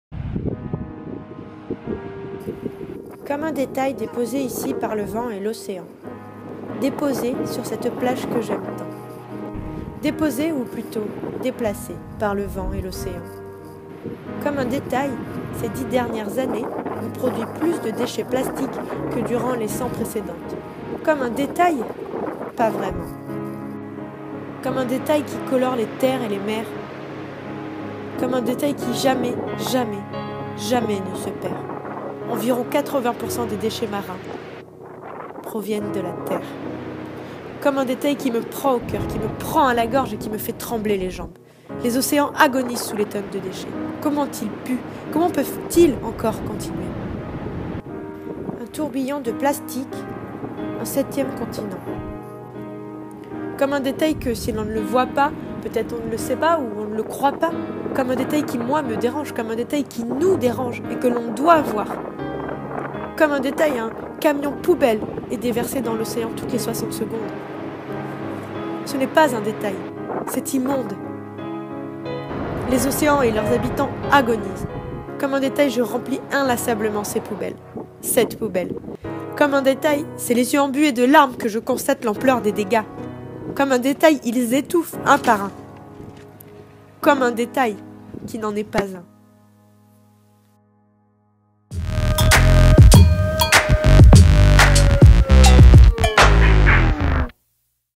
Comme un détail, qui ne l'est pas.
A toi qui ne vois que la plage nettoyé en été, à toi qui te demande si tu devrais entamer une réduction de tes déchets, à toi qui crois simplement ce que tu vois je te demande de prendre deux minutes pour regarder ça. Ouvre les yeux et réfléchis un peu. Si tu as l'impression que c'est trop tard et/ou que ce n'est pas toi qui va changer ça tout seul, imagine que tout le monde penses ça.. Alors lève toi, fais de ton mieux avec ton coeur, tes moyens et tes envies mais s'il te plait : Ouvre les yeux ! Aide tes proches, ta planète et toi même !

Chaque jours (ou presque) je viens me balader sur cette plage. A chaque balade ce détail qui n'en est pas un entache mon paysage et m'attriste. A chaque balade je ramasse inlassablement des déchets et remplie les poubelles de la ville. Ce jours là, j'ai eu envie de poser quelques images, quelques mots et quelques chiffres.

Ramasser un déchet par jour sur la plage, en foret, en ville ne prend que très peu de temps et peu aider la planète. Un petit coup de pouce qui ne te coute rien et qui sauve des vie.

Pour t'informer et aller plus loin dans ta démarche tu peux :
adhérer à des associations, des groupes locaux de protection de la nature, faire un don, soutenir une campagne,...
Ou si tu veux juste pour te renseigner je partage ici quelques associations bien connues (n'hésites pas à en laisser d'autres dans les commentaires, je mettrais la liste à jours!)

Lully

! Bien sûr, tu peux laisser un commentaire juste en bas, ça me ferait très plaisir d'échanger avec toi.

Si tu souhaites en savoir plus, rendez vous sur le blog
- Twitter et Instagram : @lemondedelully !